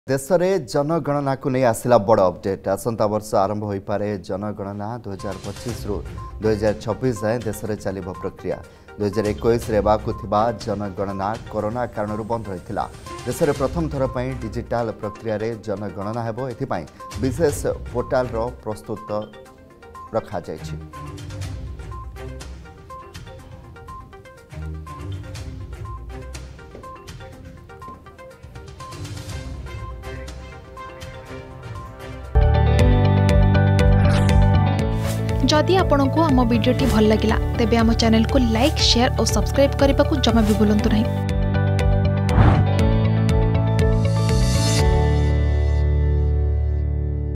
ଆସନ୍ତାବର୍ଷରୁ ଜନଗଣନା ! | Centre To Begin Census From 2025
ଦେଶରେ ଜନଗଣନାକୁ ନେଇ ଆସିଲା ବଡ ଅପଡେଟ୍ । ୨୦୨୫ ରୁ ୨୦୨୬ ଯାଏ ଦେଶରେ ଚାଲିବ ପ୍ରକ୍ରିୟା ।